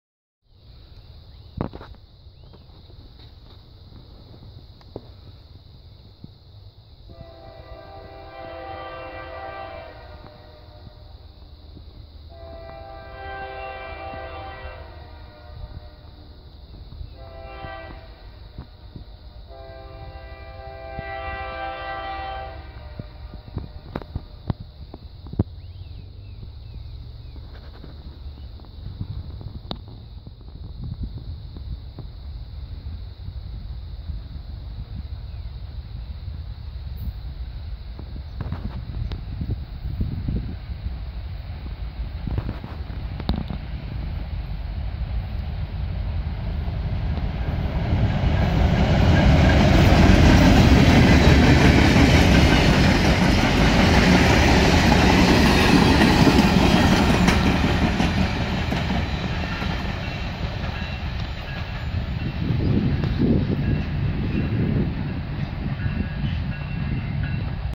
Ns Buffalo line Montandon Pa 2-23-17
North Shore -1 with nshr 1946 returning to Northumberland after working on the Union County line(UCIR)